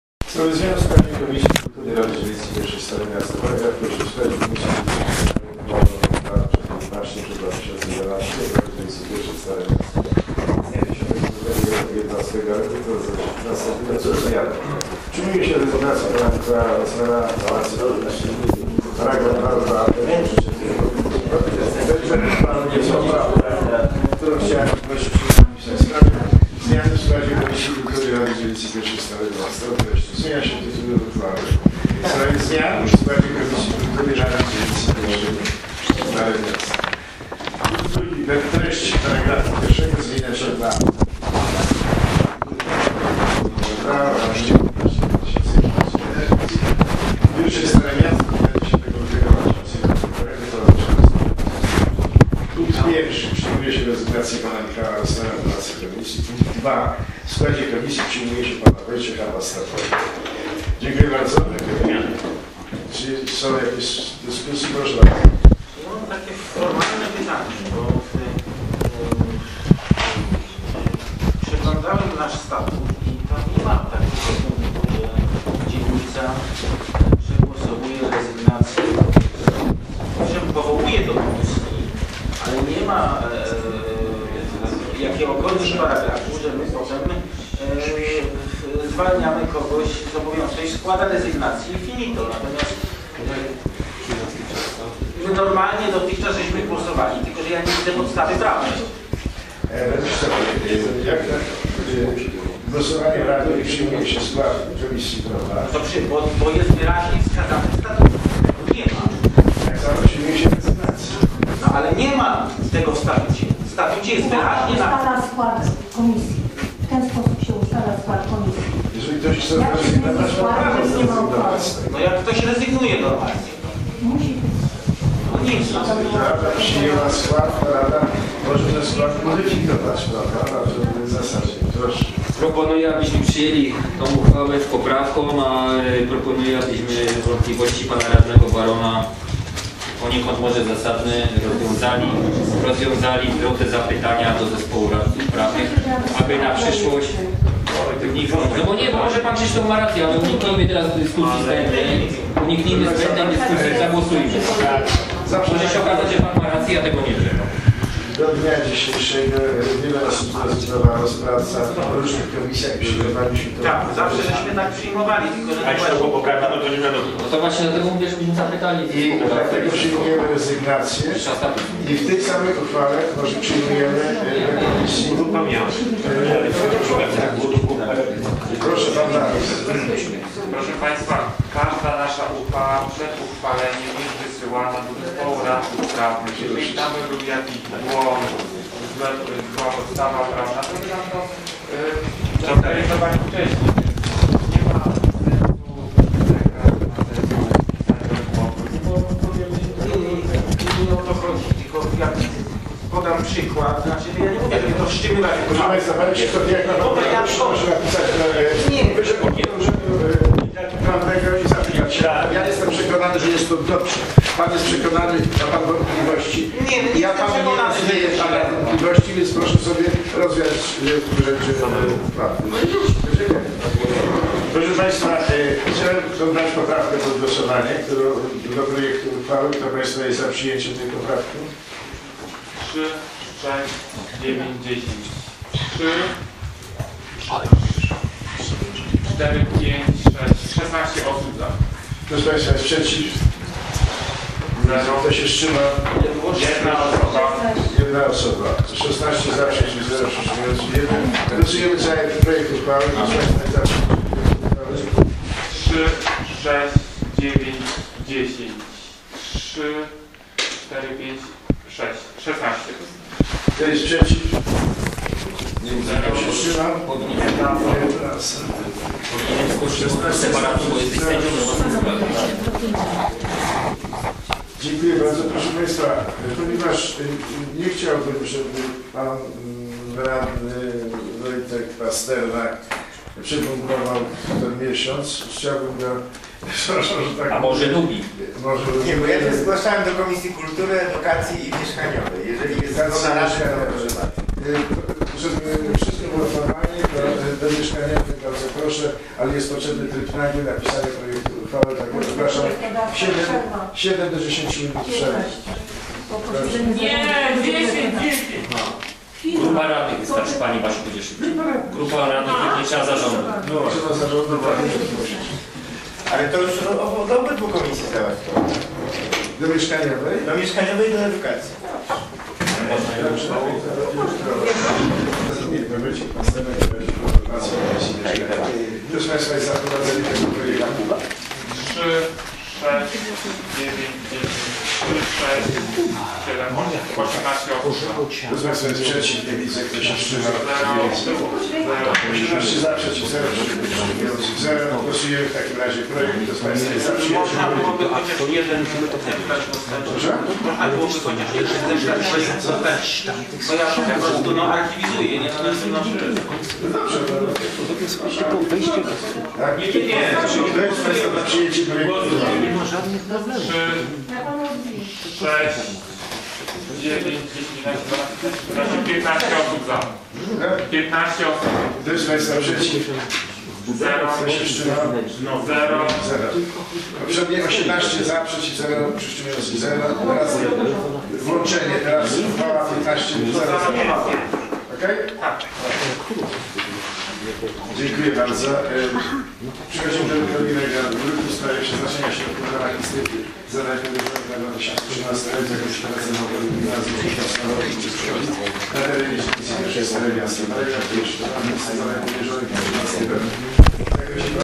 RD I 23 05 2013 8 druk 8 zmiana w Komisji Kultury
Obrady XXXIII sesji Rady Dzielnicy I Stare Miasto z dn. 23 maj 2013 r.